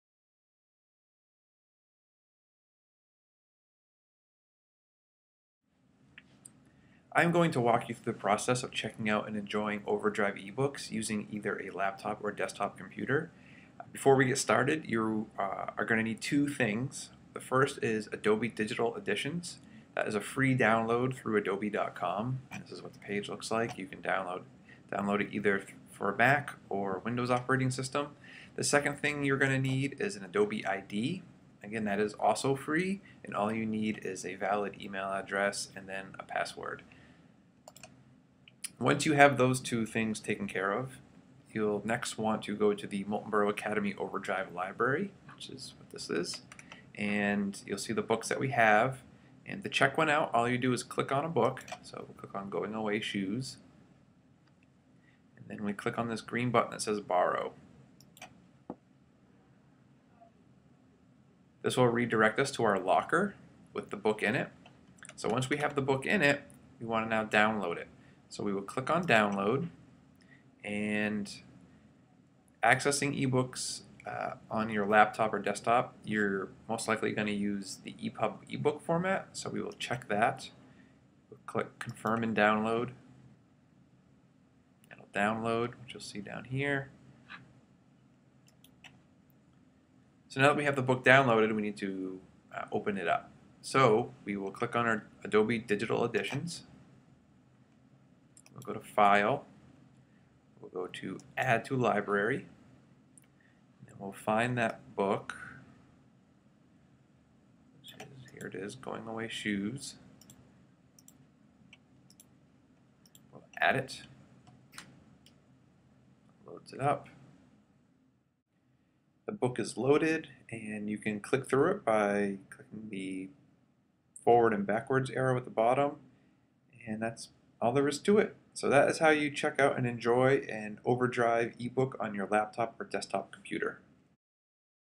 2 Minute Tutorial - Checking Out OverDrive eBooks on a Laptop or Desktop Computer
Learn how to check out OverDrive eBooks on a laptop or desktop computer.